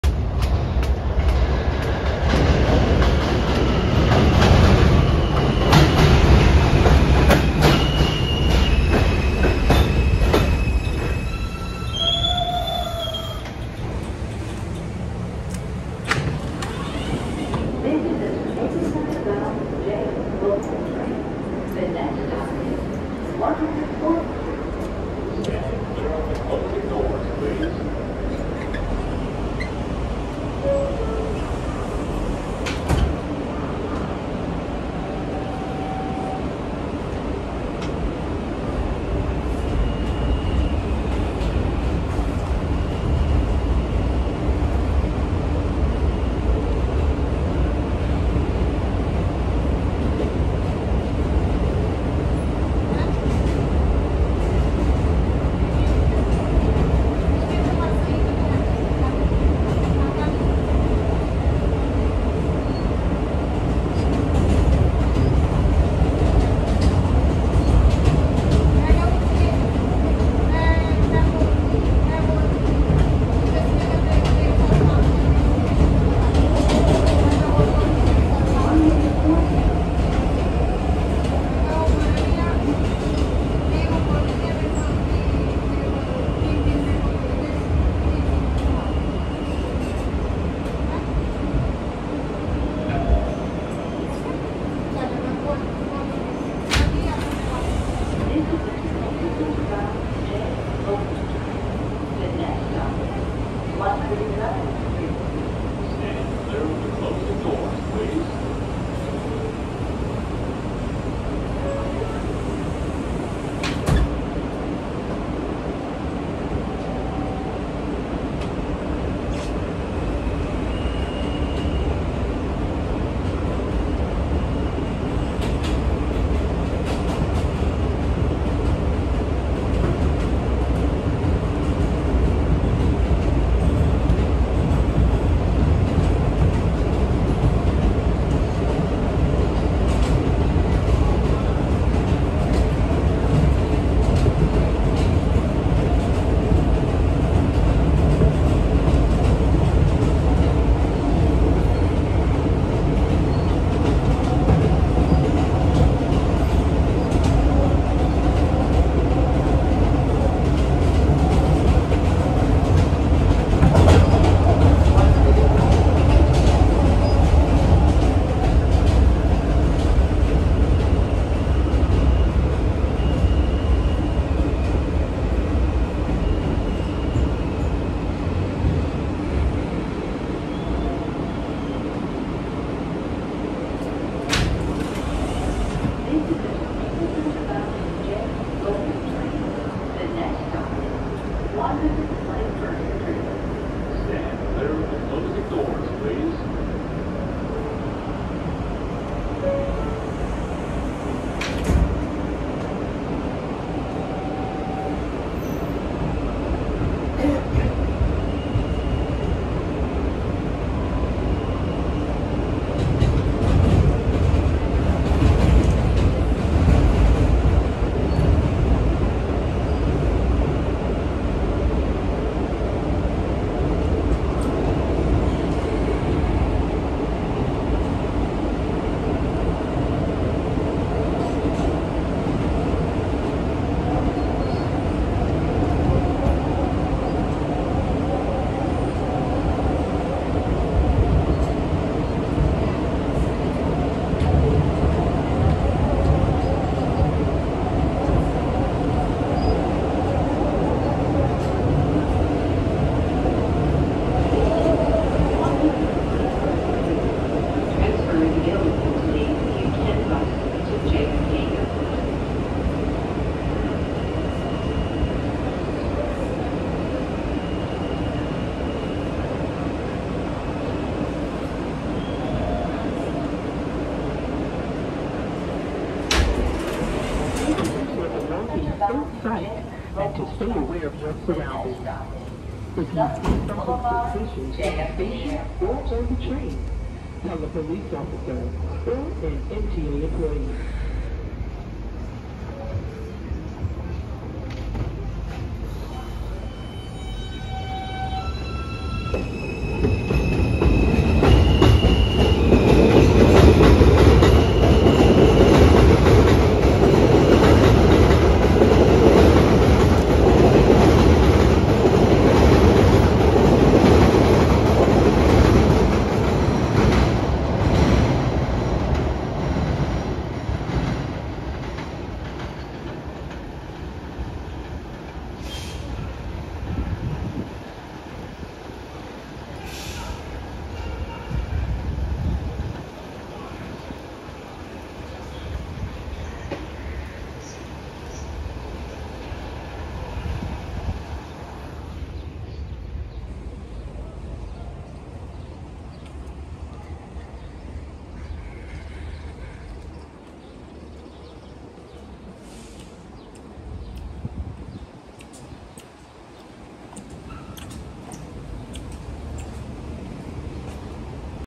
MTA R160A-1 J train full ride from Woodhaven Blvd to 121 St.
Today I went on the R160A-1 J train to 121 St.

Camera I used: iPhone 12
Date: Thursday June 2, 2022